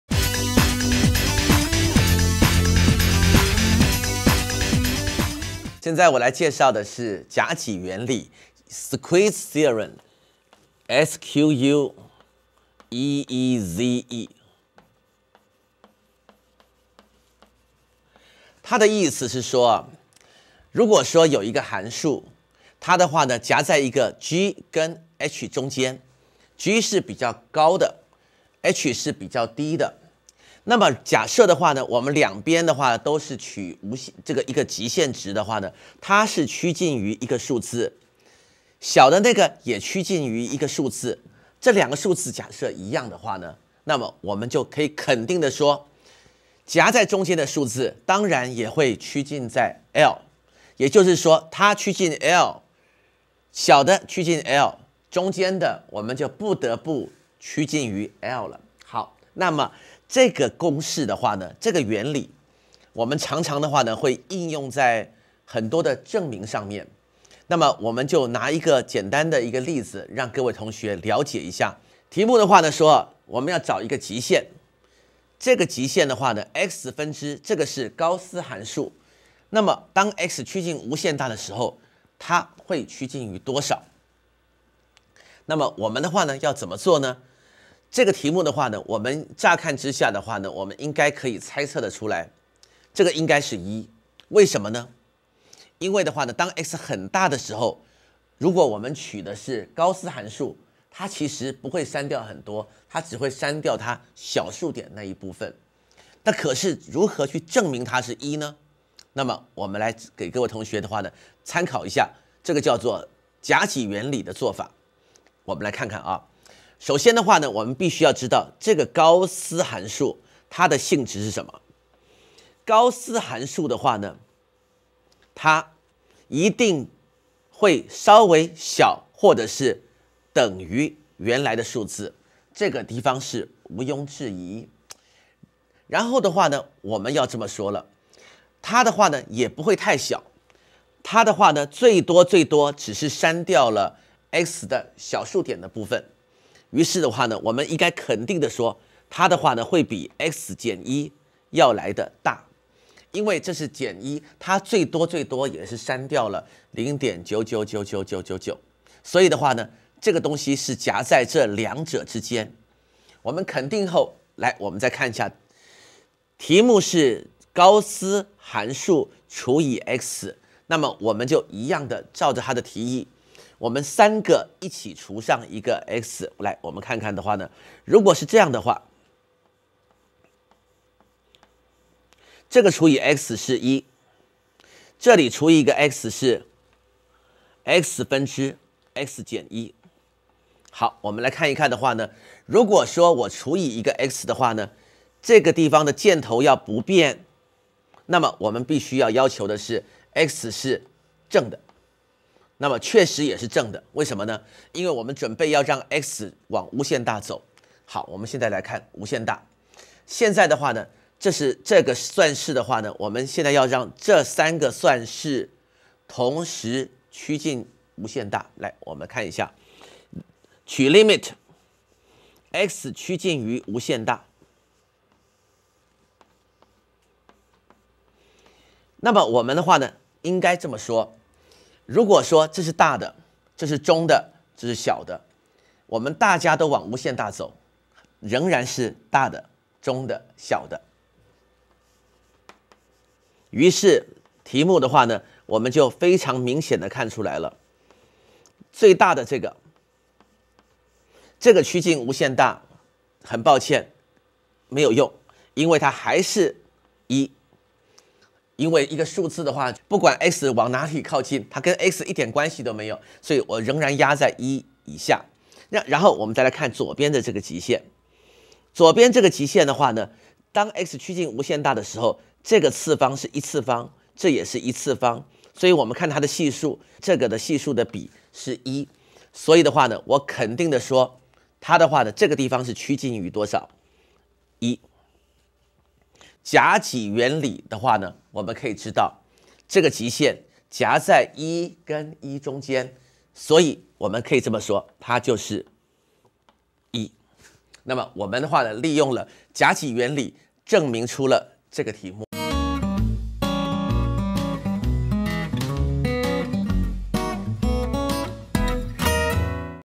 夾擠原理
課程簡介："夾擠原理"由中華科技大學李柏堅老師講授，適合剛進入大學新鮮人來觀看，內容生動又有趣，例題簡單又好記，相信同學看完之後，同學信心大增，更能了解夾擠原理的觀念。
課程難度：■□□□□
適合對象：
授課教師：李柏堅
製作單位：中華科技大學 遠距教學組
製作人員：林文博、楊智森、黃美玉、陳憶青、施驊芬、蔡鄢竹